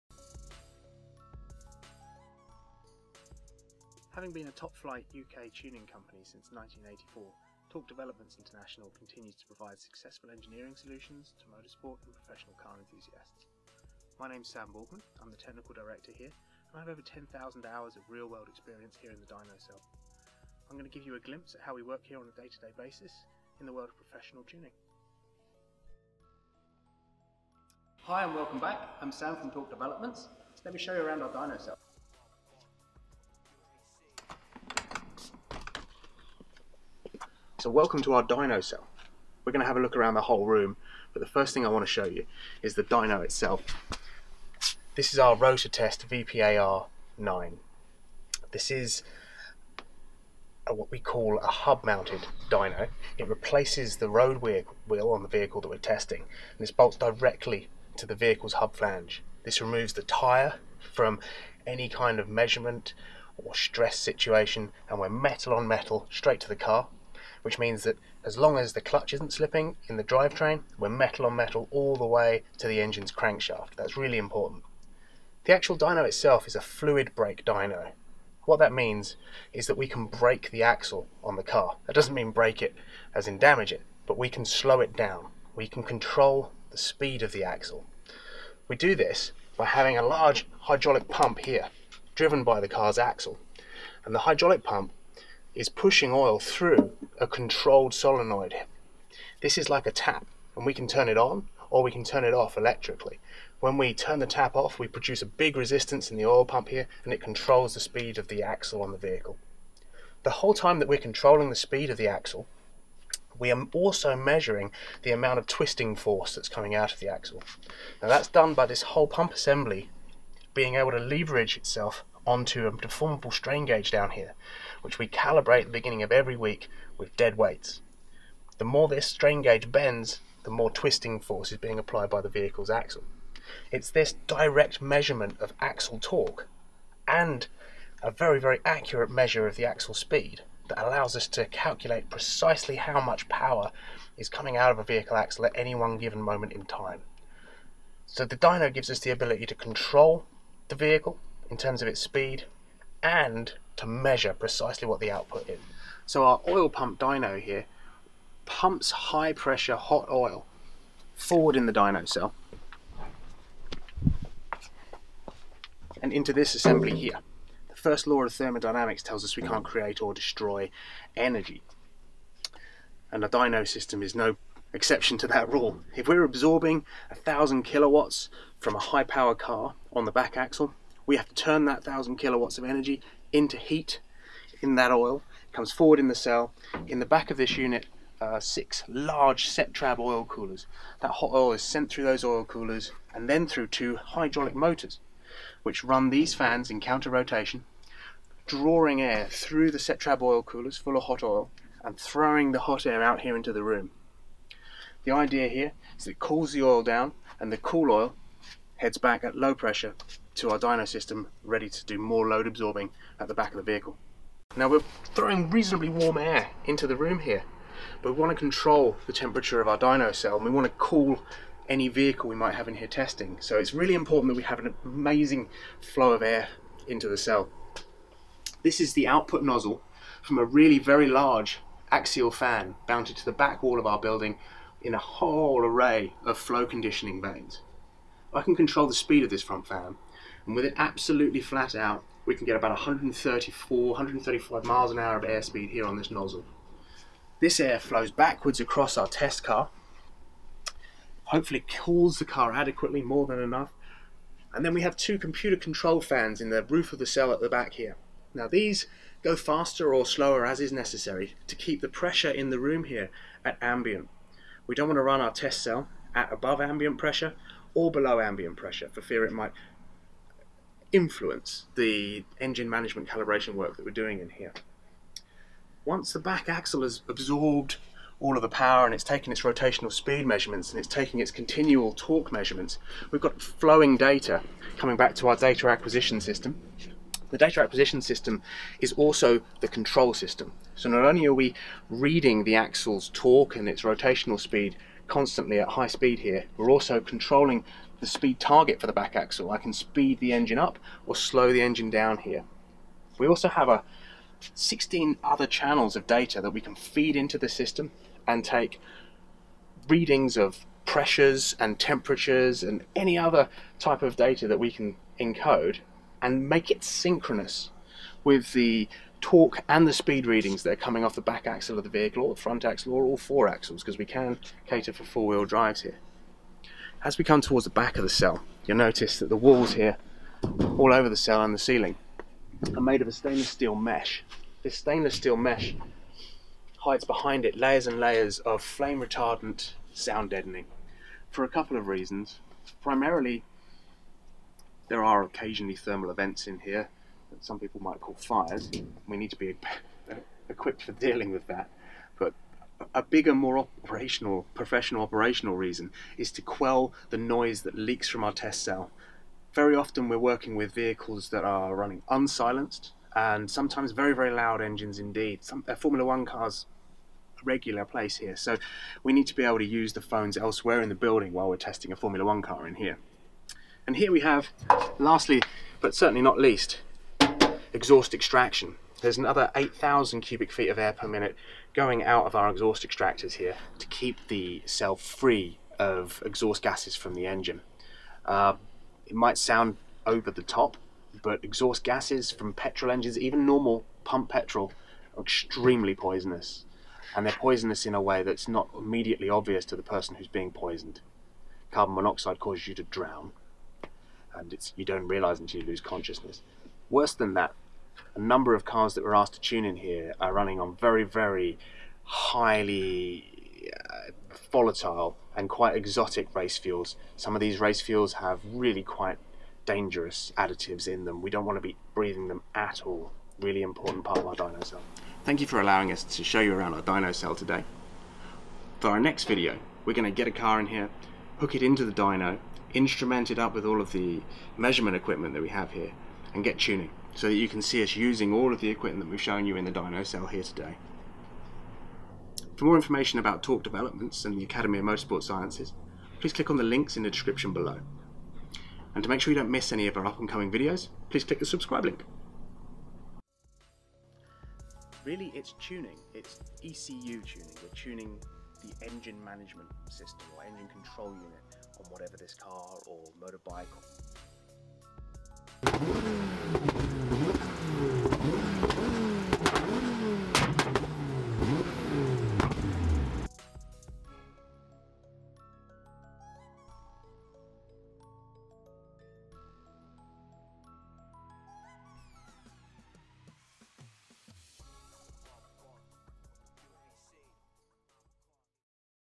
Secrets of Tuning: Inside a dyno cell
Sam Borgman, Torque Development International, takes you on a tour through their state of the art dyno cell. This rare opportunity, will give you a glimpse into UK's most respected tuning companies.

For more information about the services Torque Development offer and for more information on the Academy of Motorsport Sciences they provide please

0:44 - Look at the dyno
2:42 - Oil cooling fan
3:40 - Axial-flow fan
4:17 - Computer controlled ceiling fans
4:40 - Computer control unit
5:28 - Stainless steel mesh wall
6:13 - Exhaust extraction unit